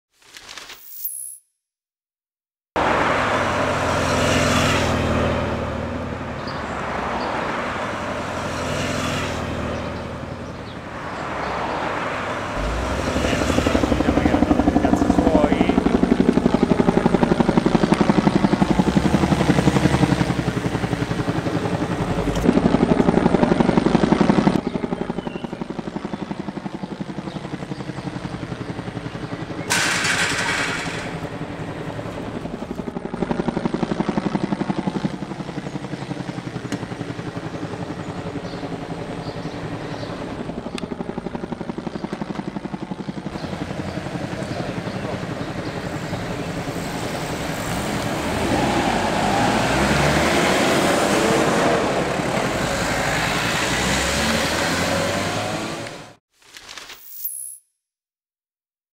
'Ndrangheta a Milano, arresti cosca Valle
Il filmato mdell'operazione della dda che il primo luglio ha arrestato 15 persone tutte accusate di associazione mafiosa.